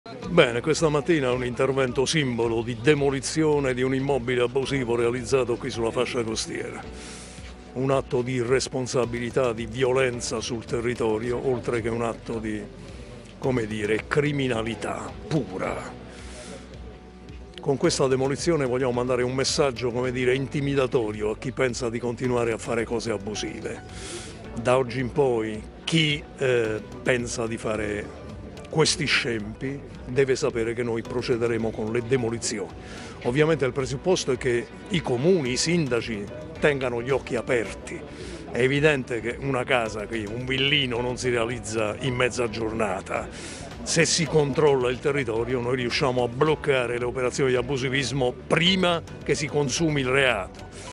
Messaggio contro l’abusivismo: demoliremo ogni scempio
Da oggi in poi chi pensa di continuare a costruire in maniera abusiva deve sapere che procederemo con le demolizioni